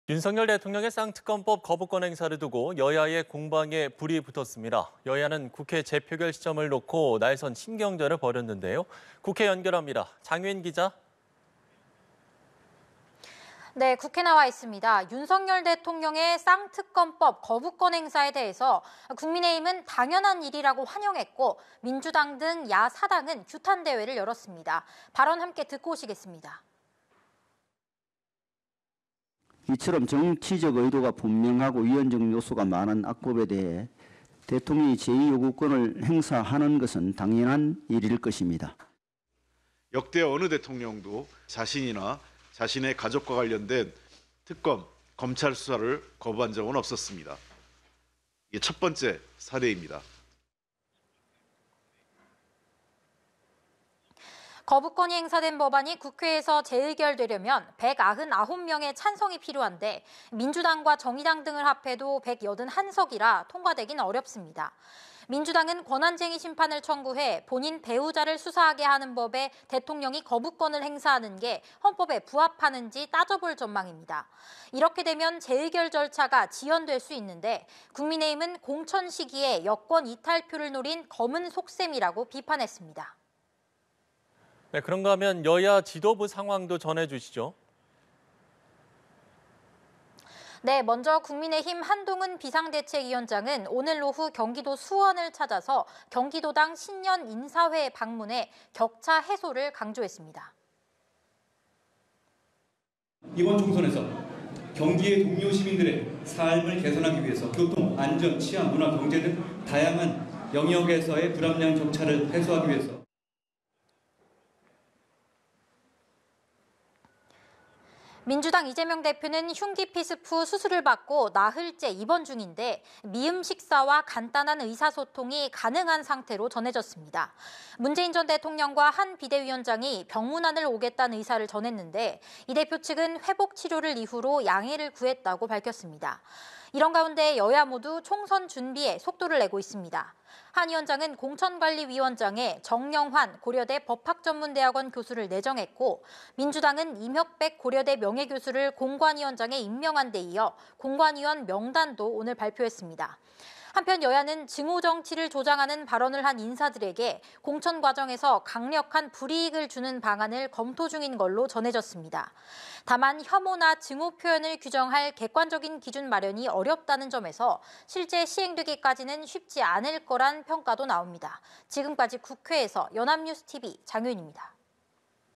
국회 돌아온 '쌍특검법'…여야, 날 선 신경전 / 연합뉴스TV (YonhapnewsTV)
국회 돌아온 '쌍특검법'…여야, 날 선 신경전

[앵커]

윤석열 대통령의 '쌍특검법' 거부권 행사를 두고 여야의 공방에 불이 붙었습니다.

여야는 국회 재표결 시점을 놓고 날 선 신경전을 벌였습니다.

국회 연결합니다.

장효인 기자.

[기자]

네, 국민의힘은 '쌍특검법' 거부권 행사가 당연한 일이라고 했고, 야권은 규탄대회를 열었습니다.

함께 듣고 오시겠습니다.

[윤재옥 / 국민의힘 원내대표] "이처럼 정치적 의도가 분명하고 위헌적 요소가 많은 악법에 대해 대통령이 재의요구권을 행사하는 것은 당연한 일일 것입니다."

[홍익표 / 더불어민주당 원내대표] "역대 어느 대통령도 자신이나 자신의 가족과 관련된 특검, 검찰 수사를 거부한 적은 없었습니다. 첫 번째 사례입니다."

거부권이 행사된 법안이 국회에서 재의결되려면 199명의 찬성이 필요한데, 민주당과 정의당 등을 합해도 181석이라 통과는 어렵습니다.

민주당은 권한쟁의심판을 청구해 본인 배우자를 수사하게 하는 법에 거부권을 행사하는 것이 헌법에 부합하는지 따져볼 전망입니다.

이렇게 되면 재의결 절차가 지연될 수 있는데, 국민의힘은 공천 시기에 여권 이탈표를 노린 '검은 속셈'이라고 비판했습니다.

[앵커]

장 기자, 여야 지도부 상황도 전해주시죠.

[기자]

네, 국민의힘 한동훈 비상대책위원장은 경기도 수원에서 열린 경기도당 신년 인사회에 참석해 '격차 해소'를 강조했습니다.

[한동훈 / 국민의힘 비상대책위원장] "이번 총선에서 경기의 동료 시민들의 삶을 개선하기 위해서 교통, 안전, 치안, 문화, 경제 등 다양한 영역에서의 불합리한 격차를 해소…"

민주당 이재명 대표는 흉기 피습 후 수술을 받고 나흘째 입원 중인데, 미음 식사와 간단한 의사소통이 가능한 것으로 전해졌습니다.

문재인 전 대통령과 한 비대위원장이 병문안을 오겠다는 의사를 전했는데, 이 대표 측은 회복 치료를 이유로 양해를 구했습니다.

이런 가운데 여야 모두 총선 준비에 속도를 내고 있습니다.

한 위원장은 공천관리위원장에 정영환 고려대 법학전문대학원 교수를 내정했고, 민주당은 임혁백 고려대 명예교수를 공관위원장에 임명한 데 이어 공관위원 명단도 발표했습니다.

한편 여야는 '증오 정치'를 조장하는 발언을 한 인사에게 공천에서 강력한 불이익을 주는 방안을 검토 중인 것으로 전해졌습니다.

다만 '혐오', '증오' 표현을 규정할 기준 마련이 어렵다는 점에서, 실제 실행되기까지 쉽지 않을 것이라는 평가도 있습니다.